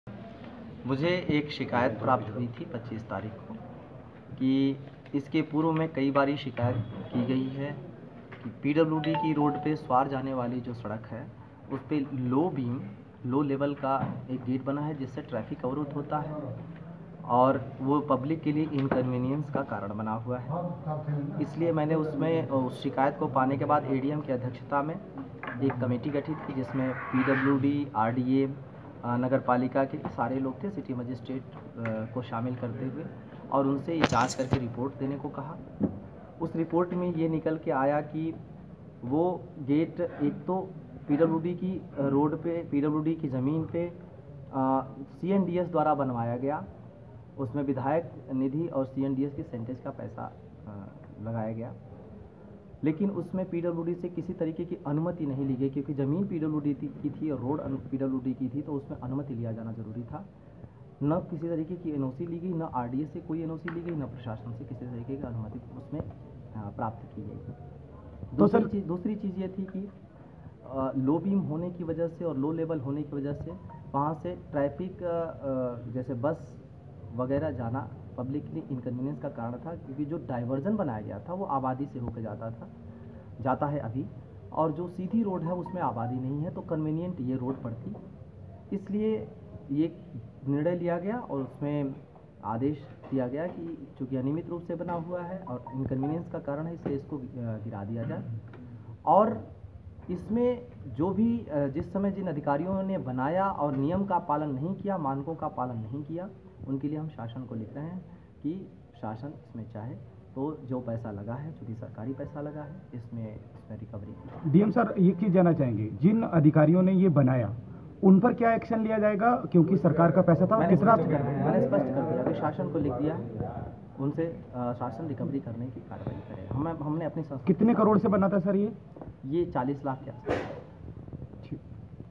मोरी गेट (उर्दू गेट) ध्वस्त करने के बाद डीएम बोले शिकायत पर की कार्रवाई
वैन (प्रियांश सक्सेना - रामपुर, उत्तर प्रदेश) :: उत्तर प्रदेश के रामपुर  जिला प्रशासन ने  भारी पुलिस फोर्स लगाकर 4 साल पुराना उर्दू गेट ध्वस्त कर दिया।  गेट समाजवादी पार्टी के शासन काल मे बना था। उर्दू गेट के तोड़े जाने को लेकर डीएम आंजनेय कुमार सिंह ने बताया कि मुझे 25 तारीख को एक शिकायत मिली थी। उस पर एक कमेटी बनाई जिसकी रिपोर्ट पर यह गेट गिराया गया है।

डीएम ने साथ ही कहा कि यह गेट पीडब्ल्यूडी  की सड़क पर बना था जो स्वार बाजपुर को जोड़ती है। शिकायत में लिखा था कि पहले भी कई बार शिकायतें की गई हैं लेकिन यह उर्दू गेट नही तोड़ा गया।  गंभीर शिकायत को जब मैने गम्भीरता से लिया तो पता चला कि यह पिछली सरकार में  40 लाख की कीमत से यह गेट सीएनडीएस कंपनी ने बनाया था।  उस कंपनी के अधिकारियों से धन की  रिकवरी  कराने के लिए भी शासन को एक पत्र लिखा है जिसमे  सीएनडीएस कंपनी से धन की रिकवरी करानी है।

डीएम आंजनये कुमार ने रामपुर में बीते 19 फरवरी को इस ज़िले की कमान संभाली थी, 25 फरवरी को उनके पास यह शिकायत आई और उन्होंने तत्काल उस शिकायत को इतनी गम्भीरता से लिया कि महज 9 दिनों के भीतर ही शिकायत पर बड़ी कार्यवाही करते हुए  उर्दू गेट को गिरा दिया ।   साथ ही डीएम ने गेट बनाने वाली कंपनी सीएनडीएस कंपनी के अफसरों से गेट को कीमत बसूल कराने के लिए शासन को एक पत्र जारी किया है । शासन के निर्देशों पर बहुत जल्द ही  उनसे धन की रिकवरी करवाई जाएगी।